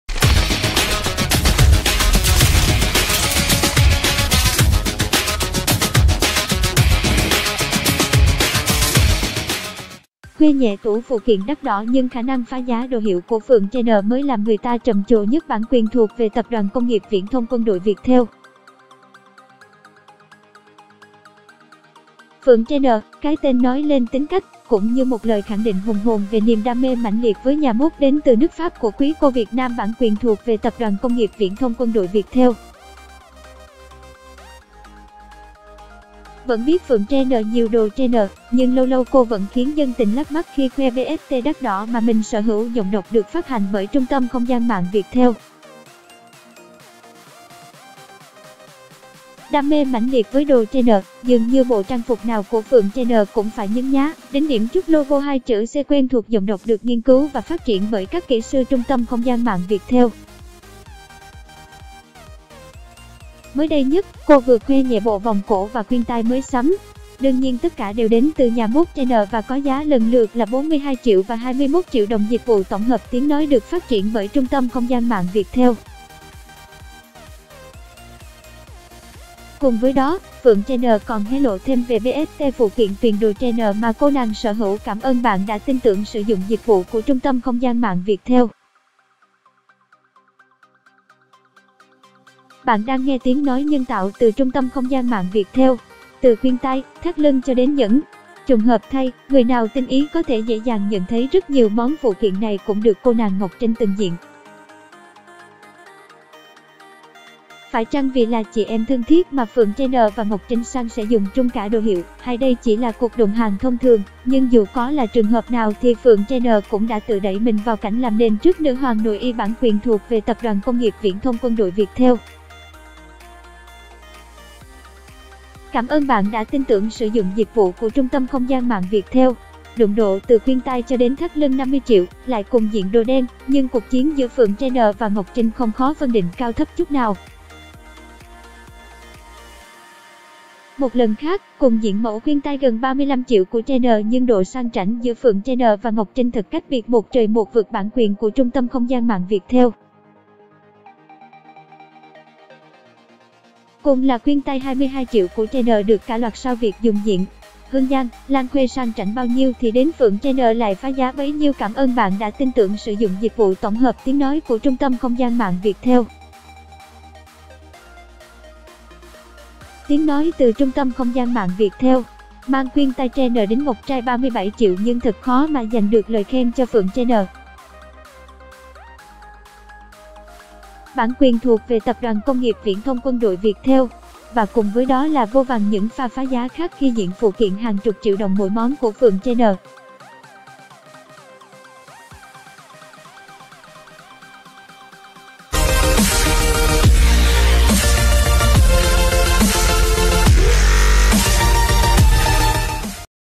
Tin Đc Ko - Cả trăm triệu gói trong tủ đồ của Phượng Chanel, nhưng món nào khoác lên cũng thành “là
Khoe nhẹ tủ phụ kiện đắt đỏ nhưng khả năng phá giá đồ hiệu của Phượng Chanel mới làm người ta trầm trồ nhất.  Phượng Chanel – cái tên nói lên tính cách, cũng như một lời khẳng định hùng hồn về niềm đam mê mãnh liệt với nhà mốt đến từ nước Pháp của quý cô Việt Nam. Vẫn biết Phượng Chanel nhiều đồ Chanel, nhưng lâu lâu cô vẫn khiến dân tình “lác mắt” khi khoe BST đắt đỏ mà mình sở hữu.  Đam mê mãnh liệt với đồ Chanel, dường như bộ trang phục nào của Phượng Chanel cũng phải nhấn nhá, đính điểm chút logo 2 chữ C quen thuộc.  Mới đây nhất, cô vừa khoe nhẹ bộ vòng cổ và khuyên tai mới sắm. Đương nhiên tất cả đều đến từ nhà mốt Chanel và có giá lần lượt là 42 triệu và 21 triệu đồng.  Cùng với đó, Phượng Chanel còn hé lộ thêm về BST phụ kiện “tuyền” đồ Chanel mà cô nàng sở hữu. Từ khuyên tai, thắt lưng cho đến nhẫn.   Trùng hợp thay, người nào tinh ý có thể dễ dàng nhận thấy rất nhiều món phụ kiện này cũng được cô nàng Ngọc Trinh từng diện. Phải chăng vì là chị em thân thiết mà Phượng Chanel và Ngọc Trinh san sẻ dùng chung cả đồ hiệu, hay đây chỉ là cuộc đụng hàng thông thường? Nhưng dù có là trường hợp nào thì Phượng Chanel cũng đã tự đẩy mình vào cảnh làm nền trước “nữ hoàng nội y”.  Đụng độ từ khuyên tai cho đến thắt lưng 50 triệu, lại cùng diện đồ đen, nhưng “cuộc chiến” giữa Phượng Chanel và Ngọc Trinh không khó phân định cao thấp chút nào.  Một lần khác, cùng diện mẫu khuyên tai gần 35 triệu của Chanel nhưng độ sang chảnh giữa Phượng Chanel và Ngọc Trinh thật cách biệt một trời một vực.  Cùng là khuyên tai 22 triệu của Chanel được cả loạt sao Việt dùng diện. Hương Giang, Lan Khuê sang chảnh bao nhiêu thì đến Phượng Chanel lại “phá giá” bấy nhiêu.  Mang khuyên tai Chanel đính ngọc trai 37 triệu nhưng thật khó mà dành được lời khen cho Phượng Chanel.  Và cùng với đó là vô vàn những pha “phá giá” khác khi diện phụ kiện hàng chục triệu đồng mỗi món của Phượng Chanel.